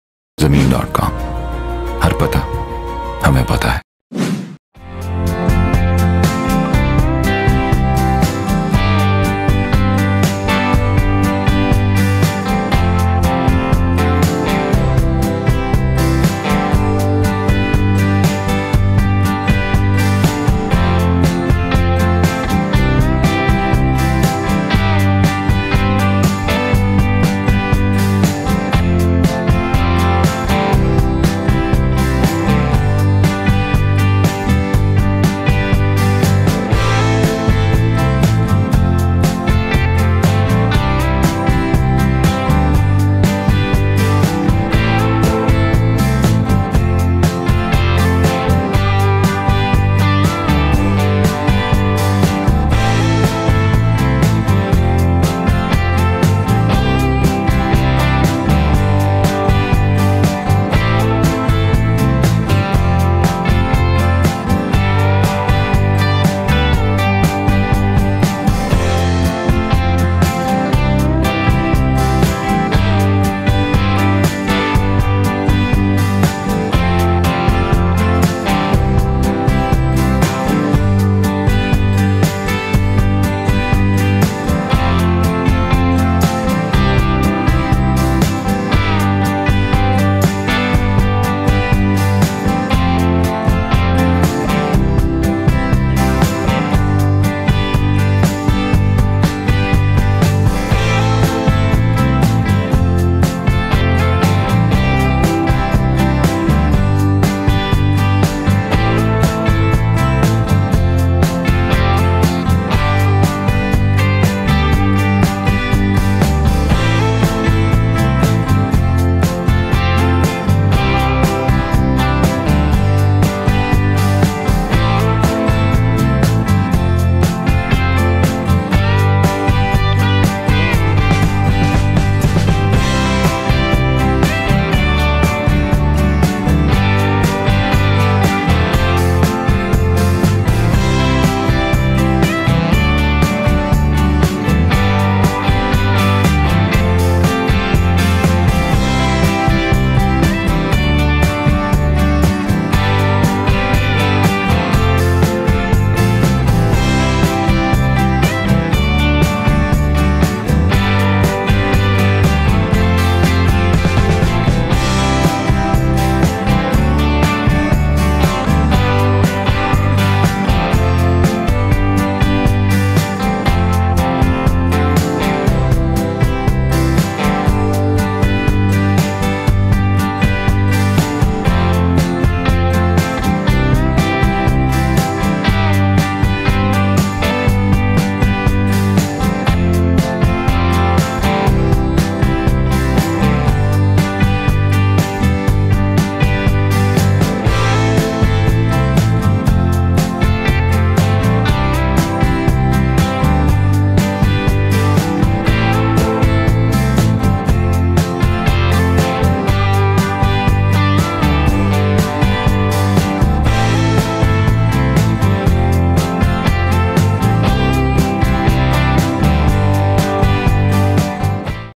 5 MARLA BRAND NEW HOUSE FOR SALE IN LAHORE MEDICAL HOUSING SOCIETY LAHORE
Khan Property Point Offers

5 Marla Brand New Beautiful Design House is available for sale in Lahore Medical Housing Society, Lahore.

Walking Distance from Park, Mosque and Commercial Market.

Situated at Such a Hot Location.

Features:
3 Bedrooms with Attached baths
Drawing
Dining
Double Kitchen
Double TV Lounge
Huge Garage
1 Powder room
1 Steady Room.